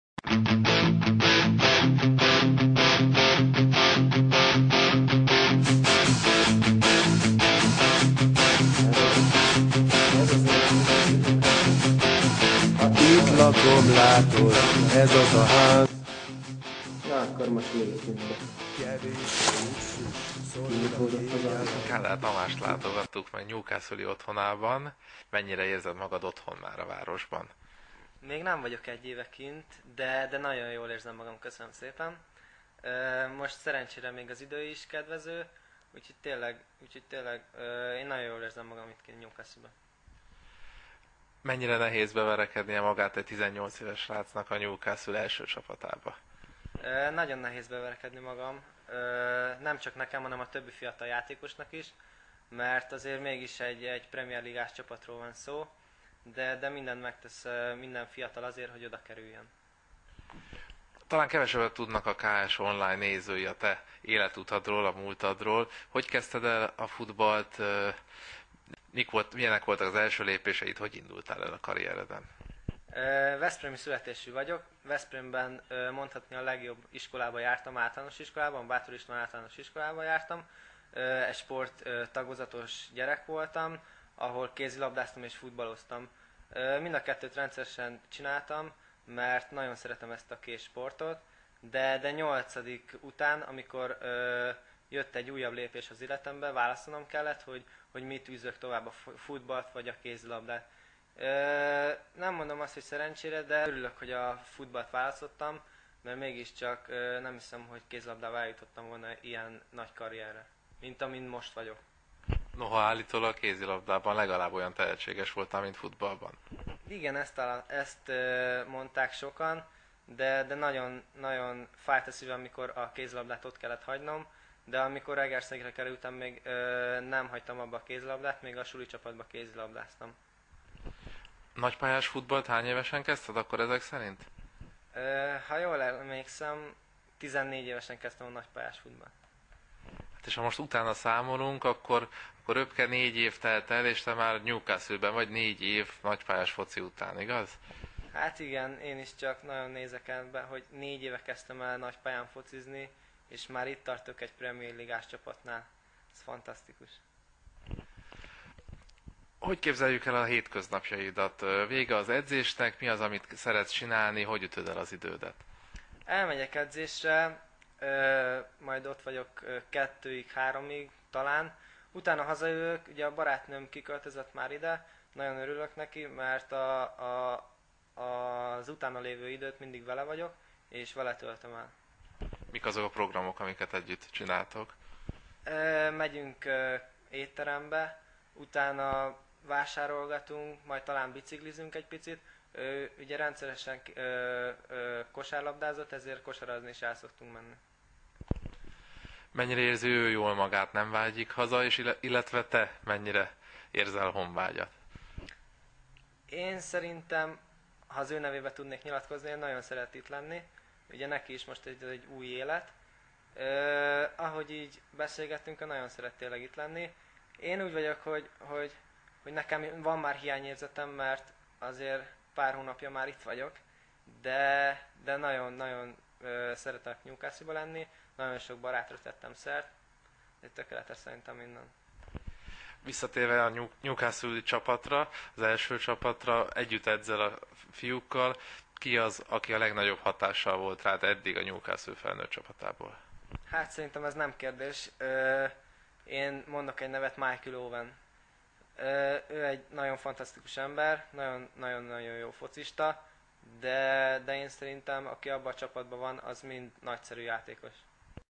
Tamas Kadar Cribs
Tamas Kadar shows us around his home in Newcastle